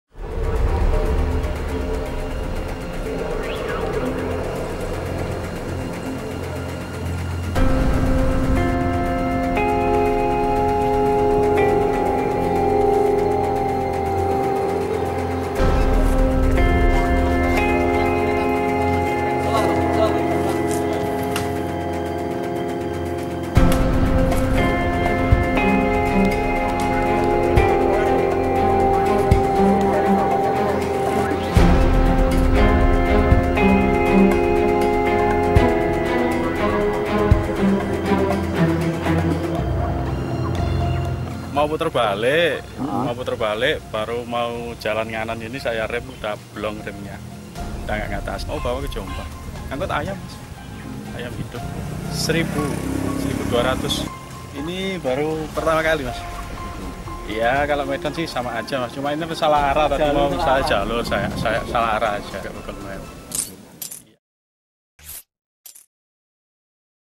TRUK MUATAN AYAM TERJUN KE SUNGAI AKIBAT GUNAKAN GOOGLE MAPS
Karena mengikuti arahan Google Maps, sebuah truk muatan ayam terjun ke sungai. Truk bertujuan ke Jombang membawa muatan ayam.

Akibat salah jalan truk hendak putar balik. Namun gagal, dan kemudian jatuh ke bibir sungai. Beruntung sopir selamat dan tidak terluka. Namun akibatnya, kerugian diperkirakan hingga jutaan rupiah.